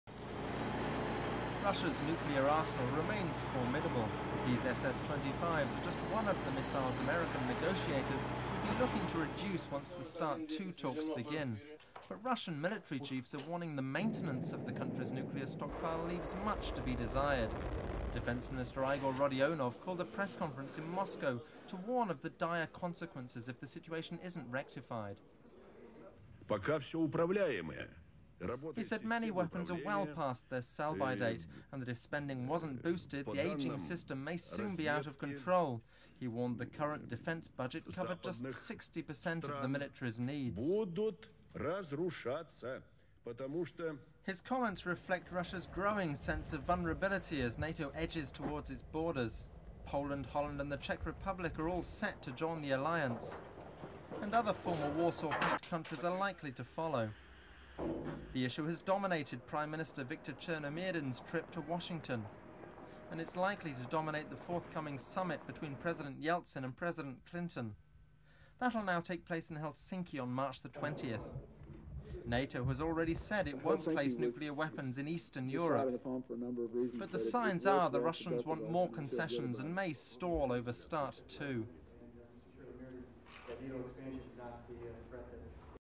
Russia/USA - Russian defence system
(7 Feb 1997) T/I:   11:17:05

Deep in its forest hide-away, a mobile single warhead SS-25 missile, Russia's key nuclear weapon, moves towards its launch pad. Reductions of these missiles and the American Minuteman--a multi-warhead missile and the Western equivalent of the SS-25--will be central to the START II talks.
But if a group of deputies in the Russian State Duma have their way,
ratification of the START II will take place only if the North Atlantic Treaty Alliance (NATO) scraps its plans to expand eastward.
Russia retains a versatile arsenal of strategic and tactical nuclear missiles a decade after bilateral arms reductions treaties between the East and West started in earnest.
Many of these missiles have recently been returned from former Soviet
satellites such as Belarus and the Ukraine.
Although Russian technology may lag behind that of the West, the nuclear
weapons in the Russian stockpile actually fit more precisely the perameters set forth by START II than do those in Western strongholds.

SHOWS:

RUSSIA, RECENT:
00.00 MS missile being driven on truck.
00.06 CU missile being driven on truck.
00.09 CU soldier inside seated a control panel.
00.12 CU soldier pressing button.
00.14 MS missile being launched.

MOSCOW, RUSSIA, 7/2:
00.18 Russian Defence Minister Igor Rodionov arriving at podium at press
conference.
00.24 CA press.
00.26 SOT, RODIONOV, RUSSIAN, warning Russian nuclear system could soon be out of control.
00.37 CA journalist, sot continues.
00.40 CU Rodionov back in view, sot continues.

RUSSIA, RECENT:
00.44 MS young Russian soldier opening gates.
00.51 MS soldiers walking along underground corridoor.
00.55 MS soldiers walking through iron door, closing it.

WASHINGTON, USA, 7/2:
00.58 MS Russian prime minister Viktor Chernomyrdin and aides arriving at White House.
01.06 MS Chernomyrdin and President Clinton seated inside White House, zooms in
on Clinton.
01.11 CA press.
01.13 CU Clinton speaking.
01.20 MS Clinton and Chernomyrdin, zooms in on Chernomyrdin.

01.26 Vision Ends.